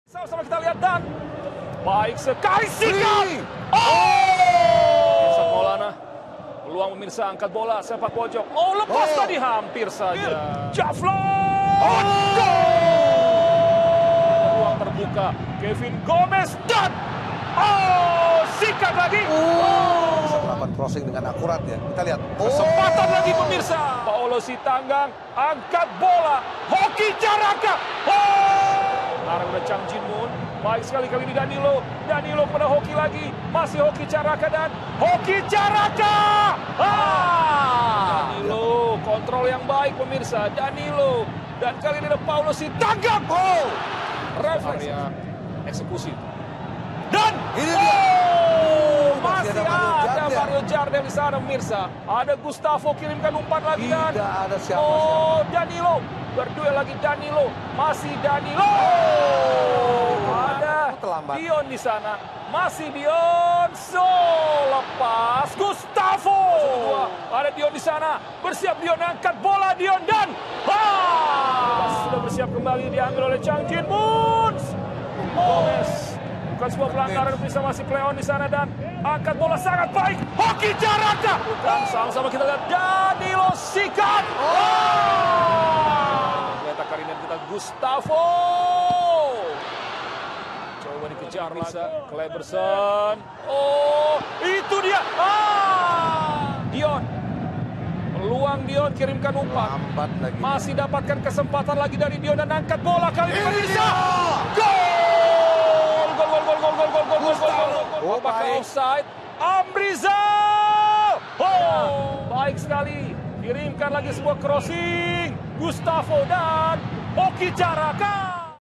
Highlight Momen: PSS Sleman vs Persita Tangerang
Highlight Momen Laga BRI Liga 1 2024/25 pekan ke-9:  PSS Sleman vs Persita Tangerang